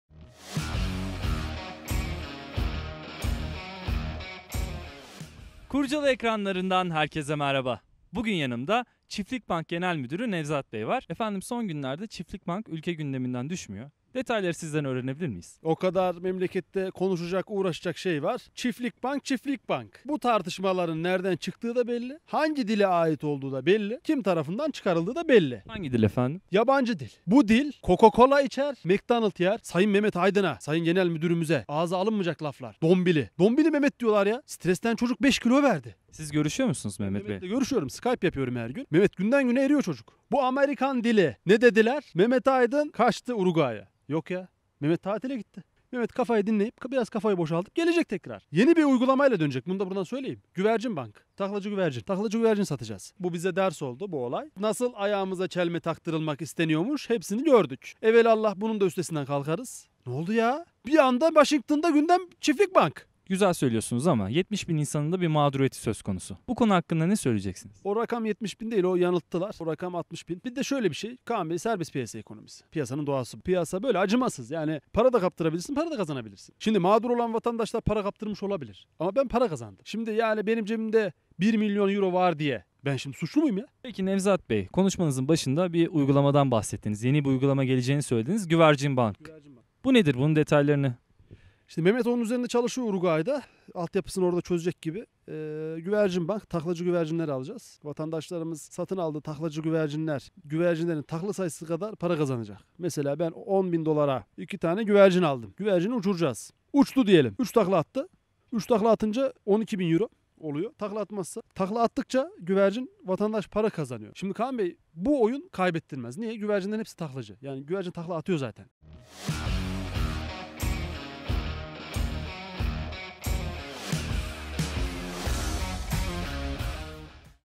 Çiftlik Bank’tan Açıklama! (Parodi İçerir)
Bu hafta bilirkişi Nevzat Can Ünsal, Kaan Kerouac'ın Çiftlik Bank hakkındaki sorularına samimi cevaplar veriyor. Çiftlik Bank tartışmalarını kim çıkarıyor? Mehmet Aydın nerede? Yeni bir uygulama geliyor mu?

►Yazan ve Oynayanlar: Nevzat Can Ünsal, Kaan Kerouac
►Kamera, Ses, Kurgu, Jenerik: Orkan Başar & Gökhan İndere